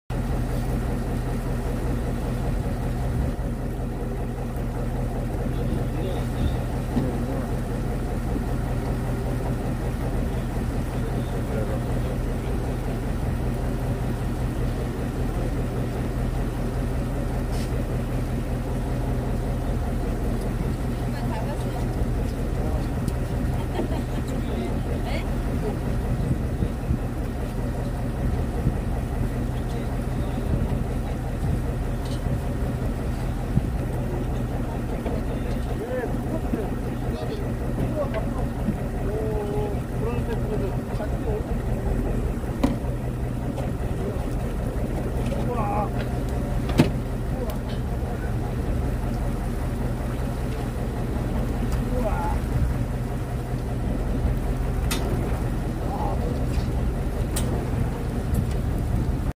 말로만 듣던 7자
소문으로만 듣던 70cm 크기 민어 잡았습니다.
낚시대 부러지는줄 알았습니다.!!!